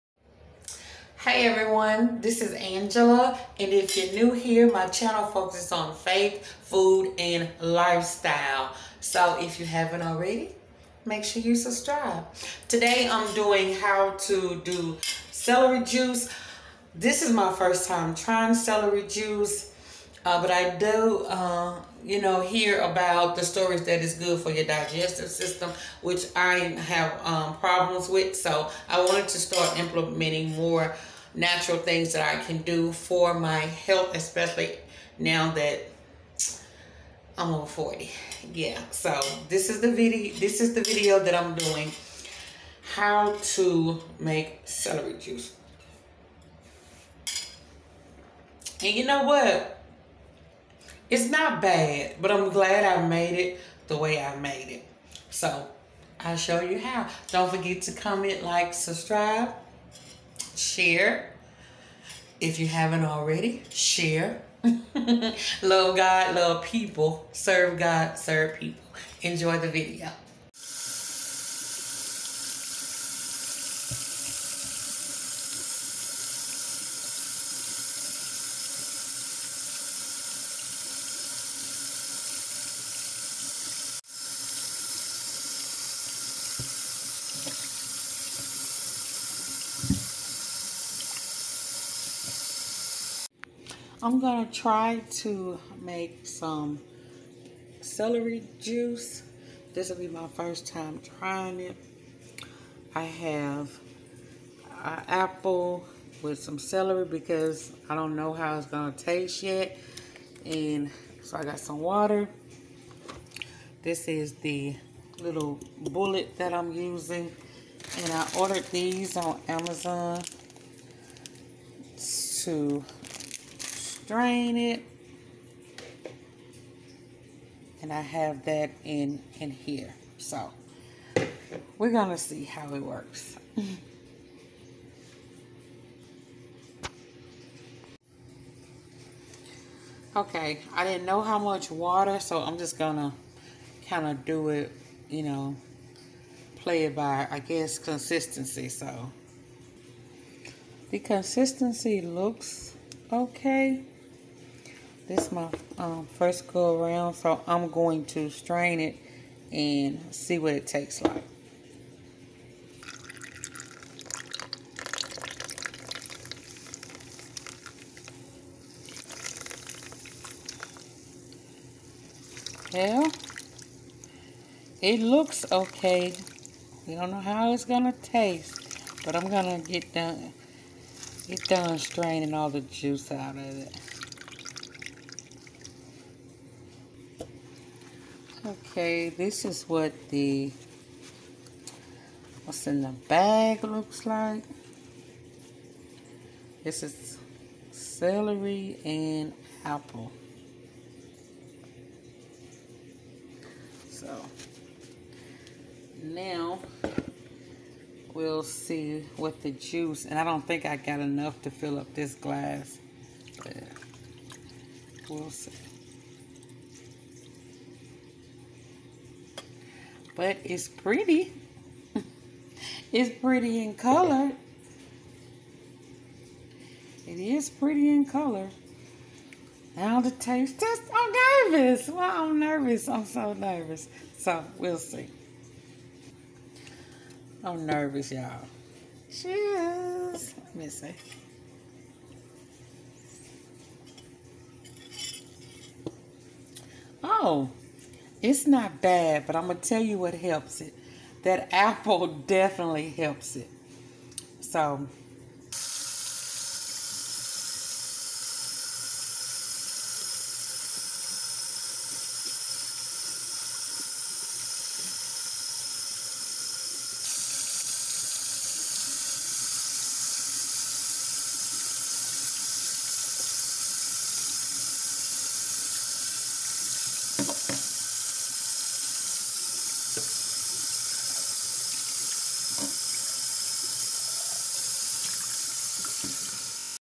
How To Make Celery Juice... Women Over 40
In today's video this was my first time making celery juice and it wasn't bad at all. I been told that there are many health benefits.